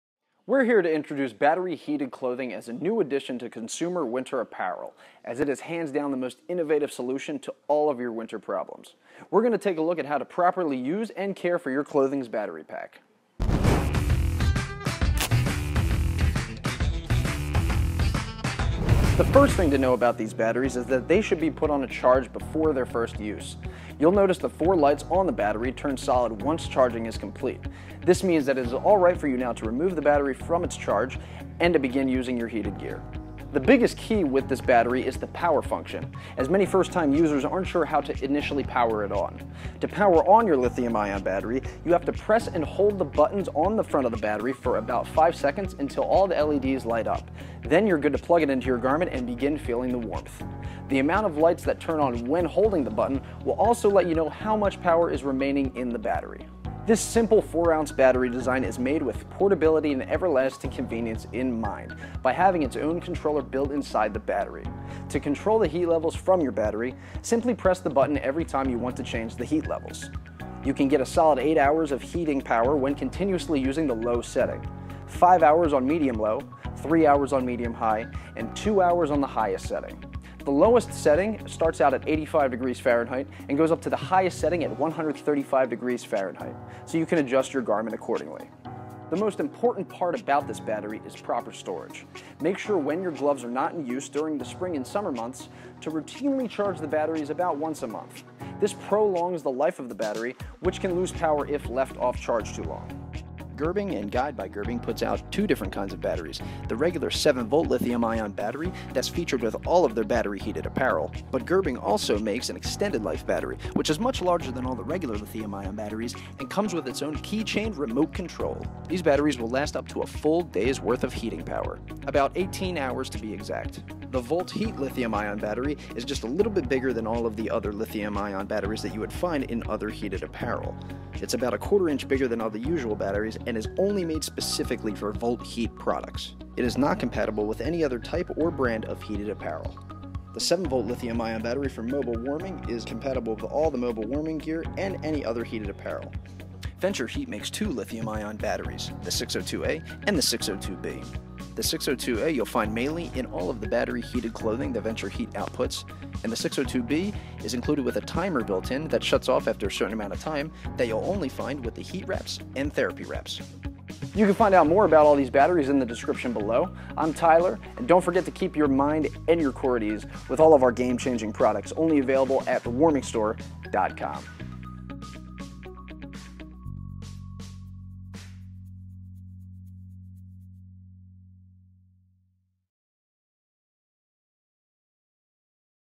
Full Lithium-Ion Rechargeable Battery Guide | The Warming Store
Battery heated clothing provides mobile warmth and comfort for a variety of outdoor winter activities. Most battery heated vests come with a rechargeable lithium ion battery that provides between 2 and 8 hours portable warmth. This is our full guide to help you maintain and care for your clothing's batteries.